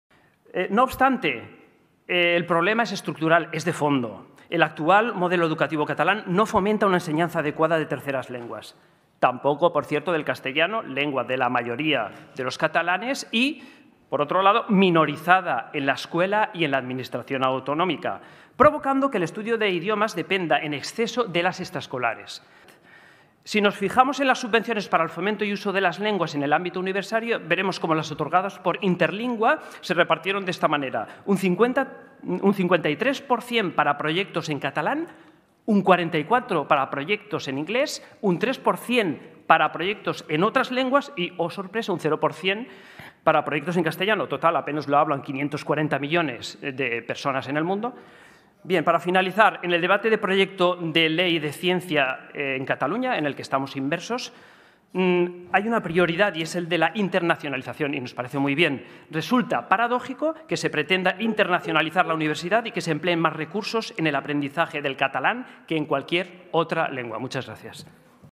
Manuel Acosta: "El modelo educativo catalán no fomenta ni las terceras lenguas ni el castellano".
Manuel Acosta en el pleno del Parlament sobre la exigencia de acreditar terceras lenguas para obtener el título universitario:

"Las subvenciones en el fomento de las lenguas en ámbito universitario son de 53% para el catalán, 44% para el inglés, 3% para otras lenguas y 0% para el castellano. Total, apenas lo hablan 540 millones de personas en el mundo"